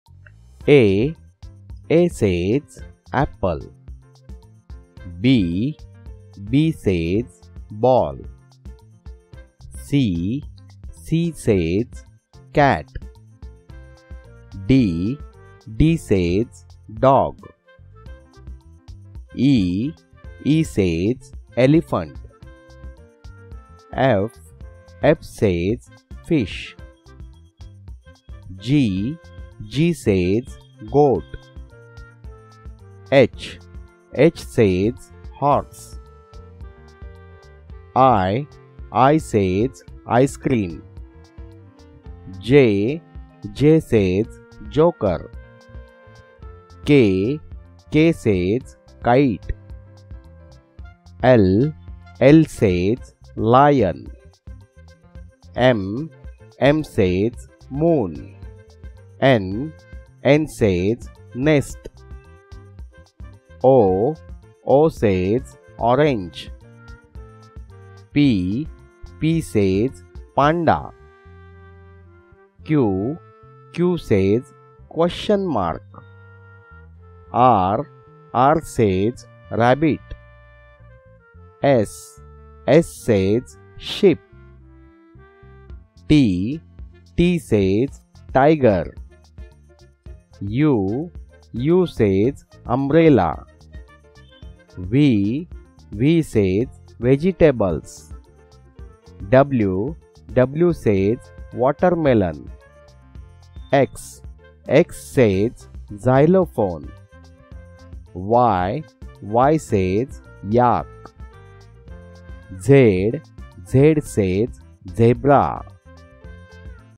Learn Alphabet A to Z | ABC for Preschool
A says Apple B says Ball, English alphabet Abc phonics with video image.
In this video, we will learn how to say letters from A to Z Uppercase and Smallcase and also see each letters live defination.
Kids will learn Alphabet and its video with pronunciation.
ABCD Alphabets for kids by Ajinkya Innovations.